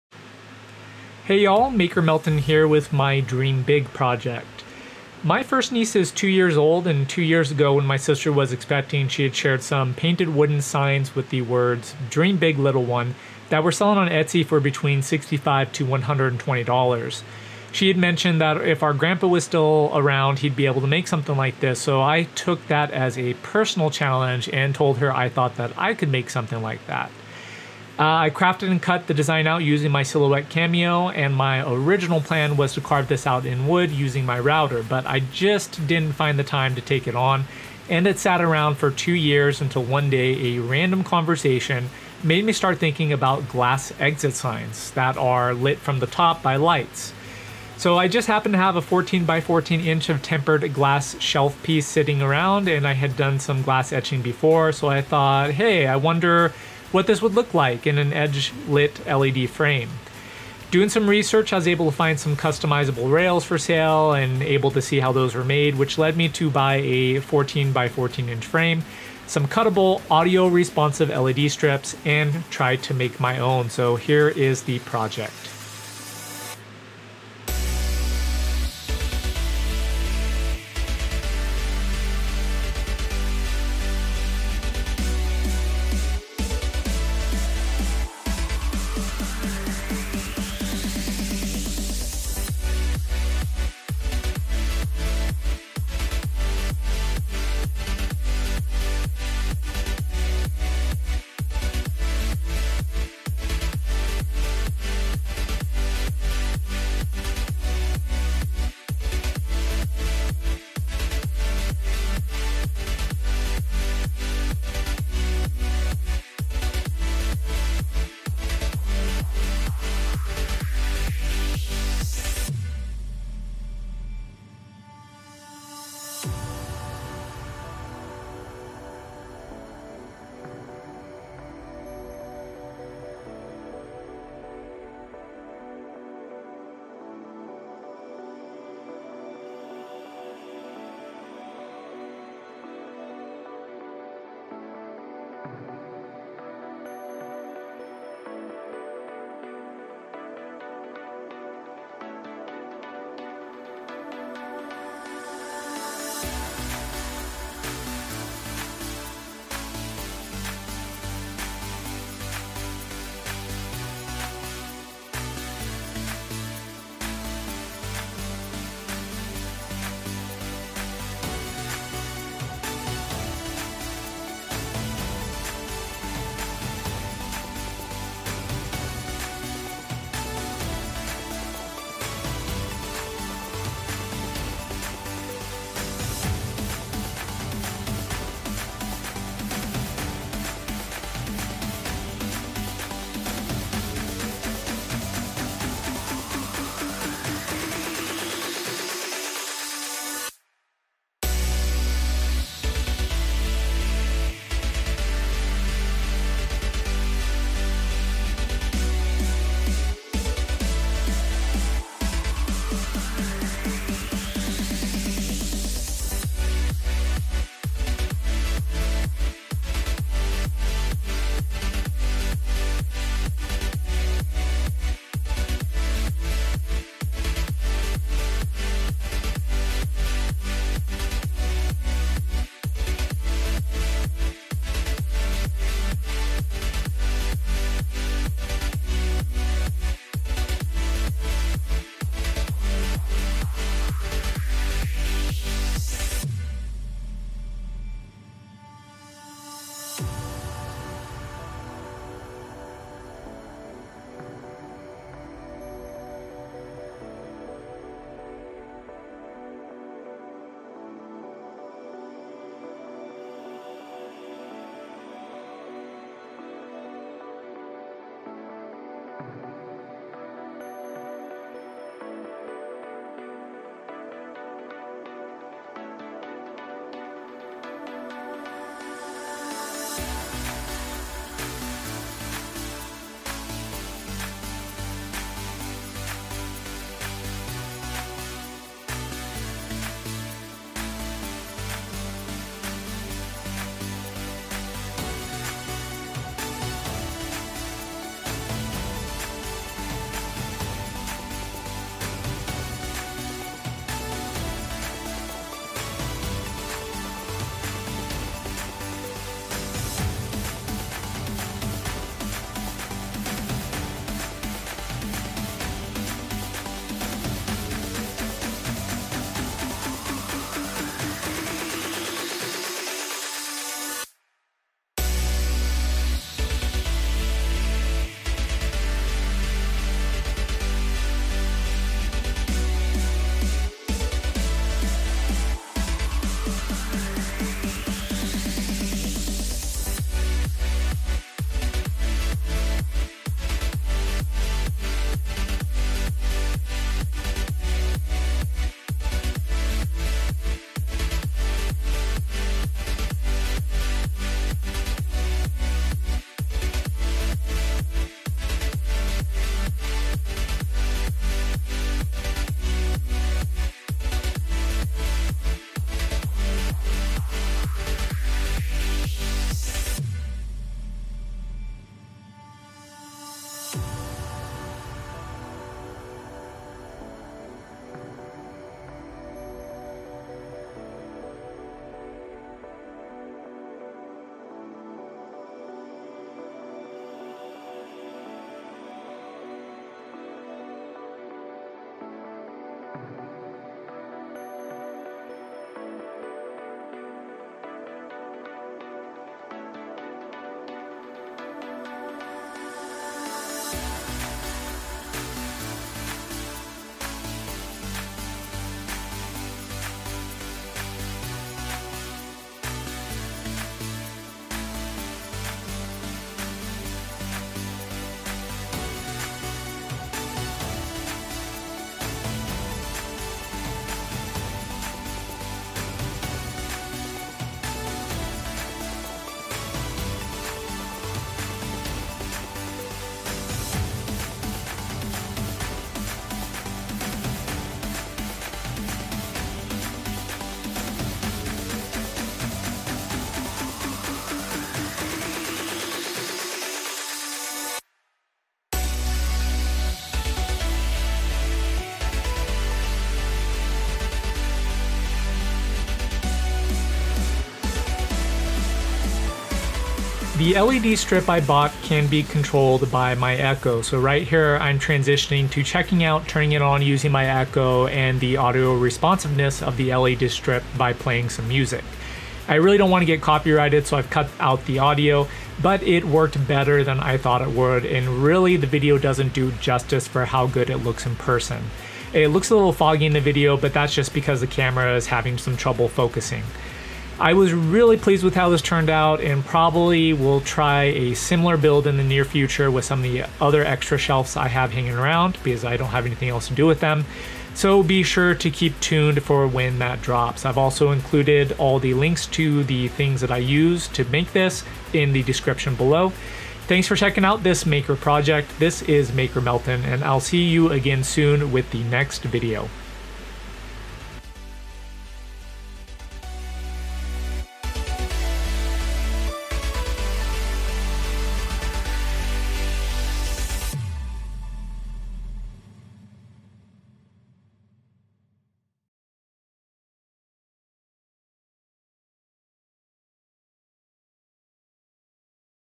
Making the Dream Big Glass Etching in LED Edge-lit Frame
This was a project that took me 2 years to get to, and it ended up being a very different project from the original idea of a wood carving! The idea came to me when a random conversation made me think of glass exit signs that are lit from the top.

So why not create my own edge-lit LED frame to see if I could get a similar effect on a glass etching?

Main Supplies:

Armour Etch Etching Cream

Wood Float Frame by Darice 14"x14" picture size - bought at wayfair.com

Govee Alexa LED Light Strip

Tools:

Silhouette Cameo 4, phillips screwdriver, Dremel tool (w/drum sander sleeves and mandrels), digital caliper, medium grit sandpaper, tape, paintbrush